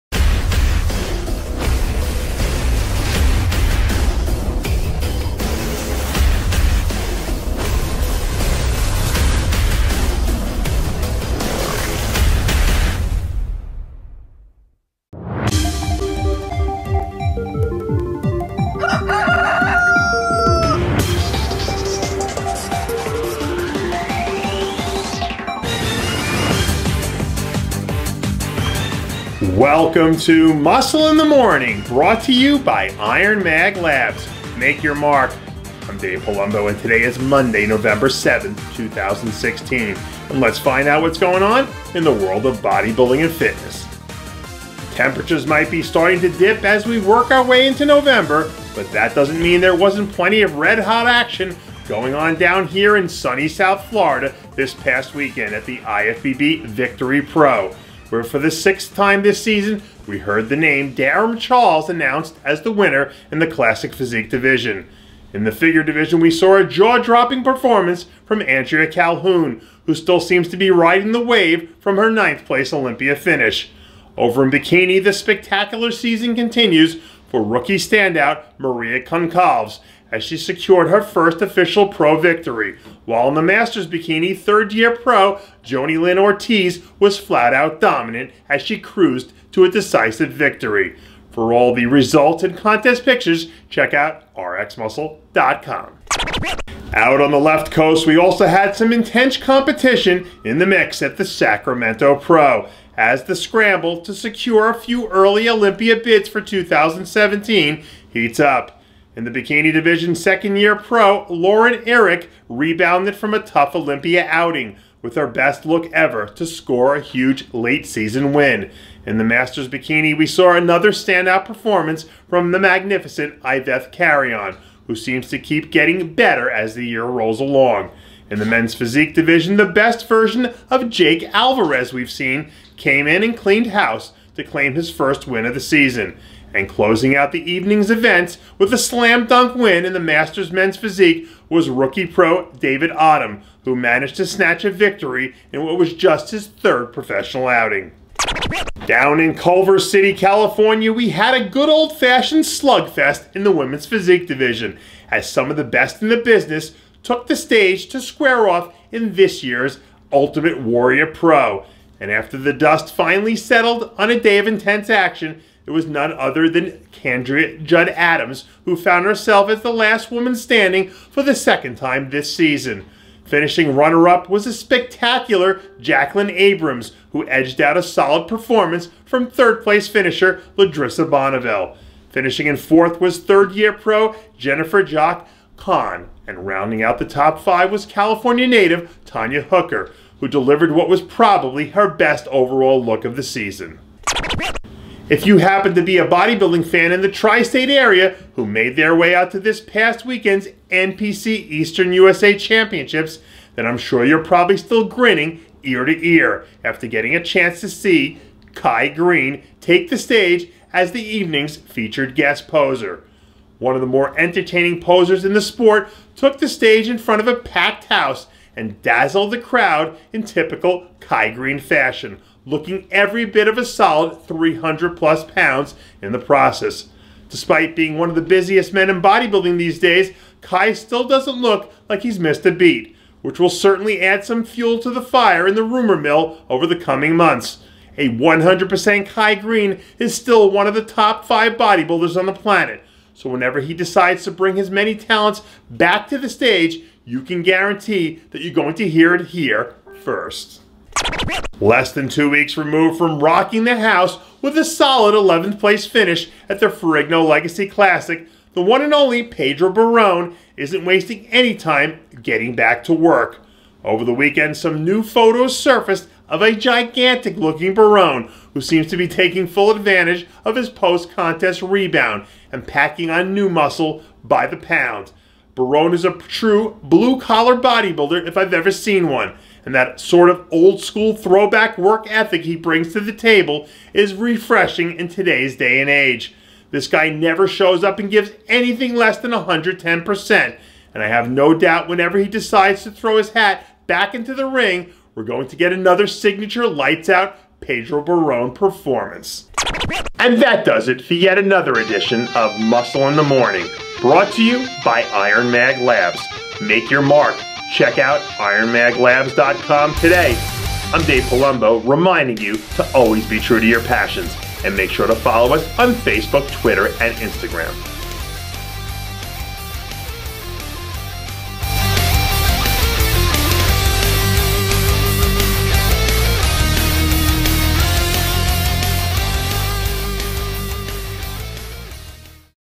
KAI GUEST POSES IN NY! - Muscle In The Morning November 7, 2016
Florida Victory Pro Recap, Sacramento Pro Recap, Ultimate Warrior Pro Recap, Kai Greene Guest Posing, Pedro Baron Back to Work